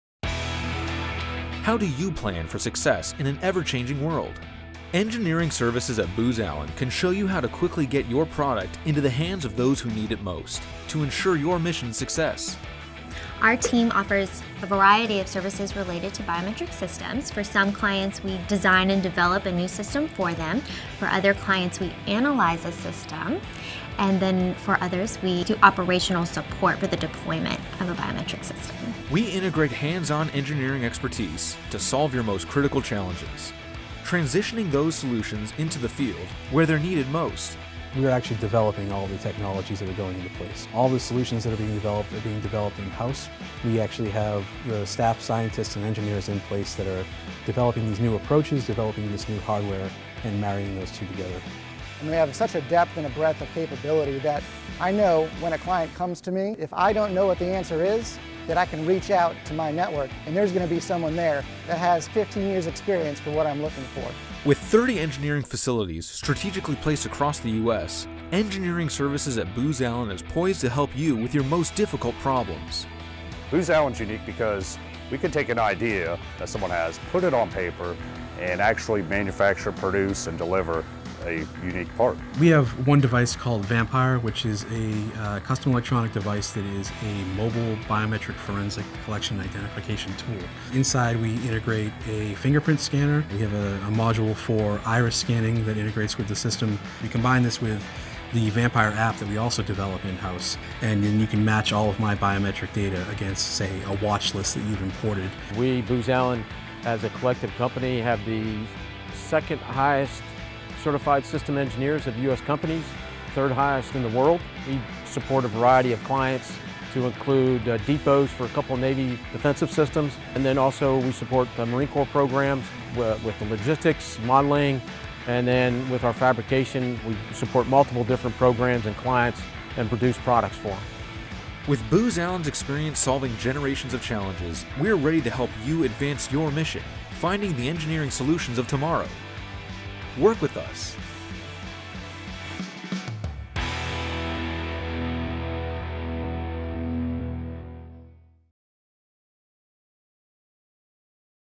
Booz Allen Engineering Your Mission’s Success
Booz Allen integrates hands-on engineering expertise to solve the toughest problems for civilians, soldiers and citizens. With 30 engineering facilities, we can design, manufacture, and deliver the solution. From biometrics systems to systems engineering, we find the engineering solutions of tomorrow.